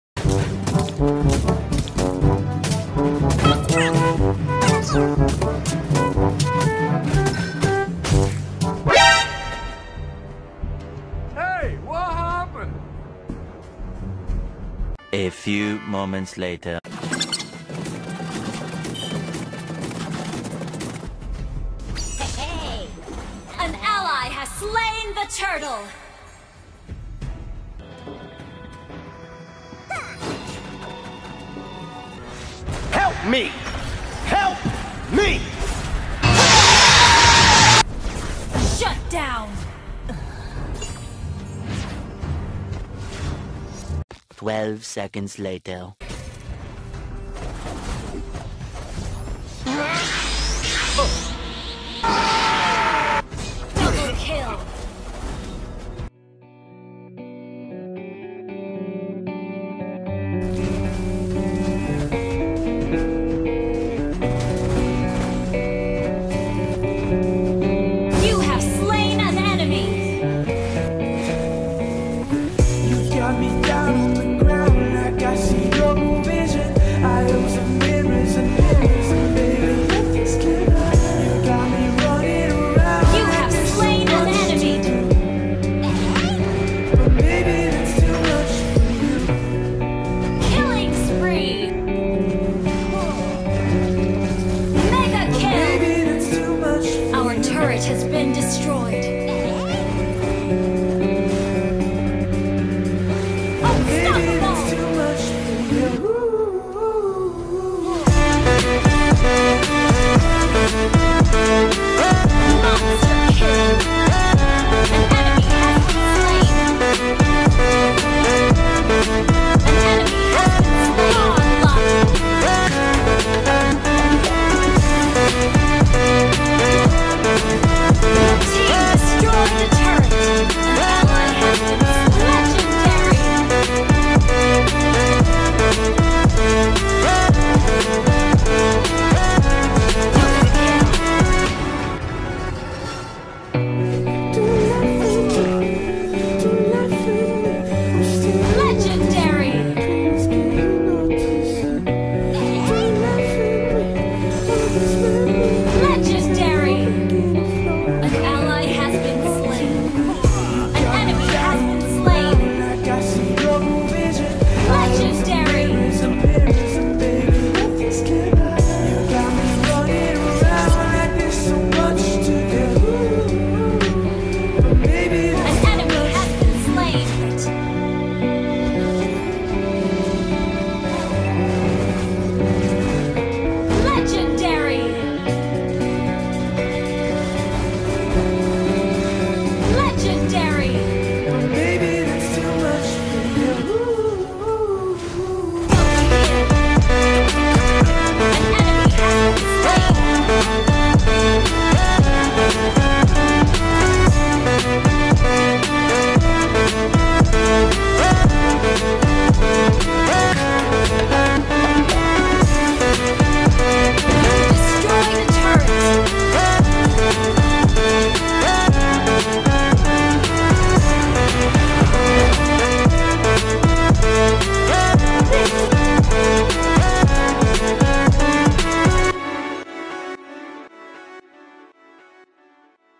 harley build season 13
season 13 and the latest patch made harley so weak. with fast mana drain..and low damage.. but.. with this build makes harley so effective early game and with the right amount of push and farm its a sure win.
check the thumbnail for the build. with custom mage emblem.